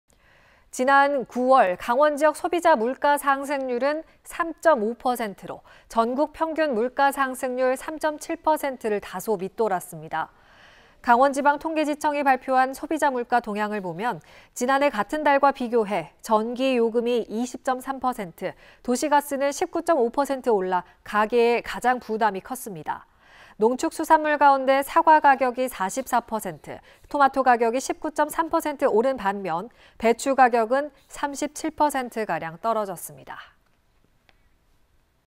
2023. 10. 5 [원주MBC] 9월 강원 소비자물가 3.5% 올라
지난 9월 강원지역
소비자 물가상승률은 3.5%로
전국 평균 물가상승률 3.7%를
다소 밑돌았습니다.

강원지방통계지청이 발표한
소비자물가동향을 보면
지난해 같은 달과 비교해
전기요금이 20.3%, 도시가스 19.5% 올라
가계에 가장 부담이 컸습니다.

농수축산물 가운데
사과 가격이 44%,
토마토 가격이 19.3% 오른 반면
배추 가격은 37%가량 떨어졌습니다.